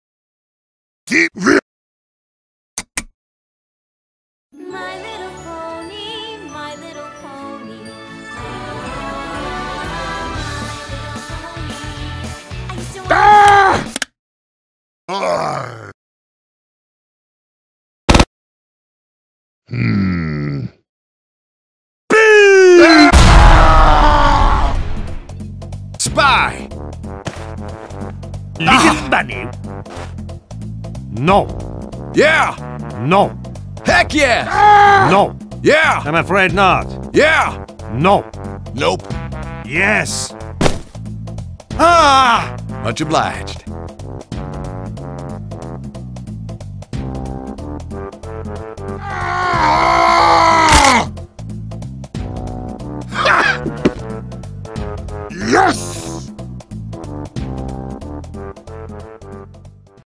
[LP] XxRiCkYFLXxX75's Hat
Important Update! Description Below:

Update: I have made a slight policy change on the [P] system. If there is any references of any sort to mlp, it will have the [LP] sign standing for Little Pony. This means that If I want to reference music, trailers, small stuff like that, then I will label all them with [LP].

Hey guys whats up its infomanica here with a brand new film. this was requested from XxRiCkYFLXxX75 and I did my best to give him what he wanted. His editor broke so he can no longer make films... but he said that his character was a heavy with the fancy fedora so I went with that. hope you all like it!